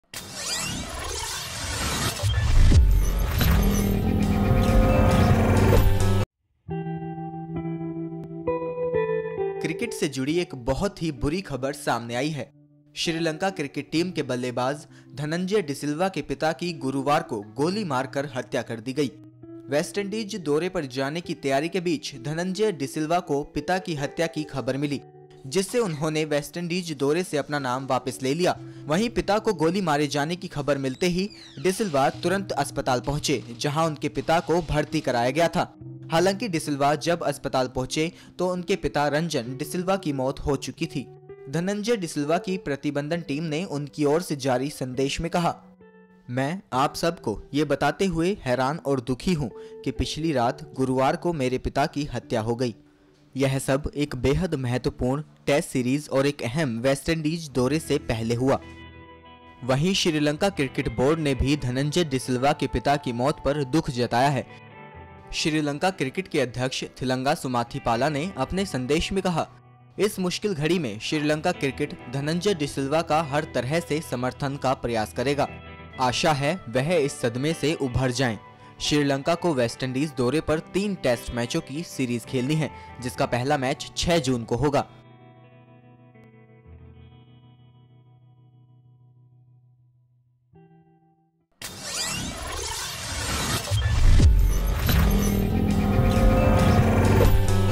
विदेश दौरे से पहले इस Player को मिली पिता के Murder की खबर
Sri Lanka के Batsman Dhananjay De Silva के पिता का गोली मारकर Murder कर दिया गया, जिससे West Indies दौरे से अपना नाम वापस लेना पड़ा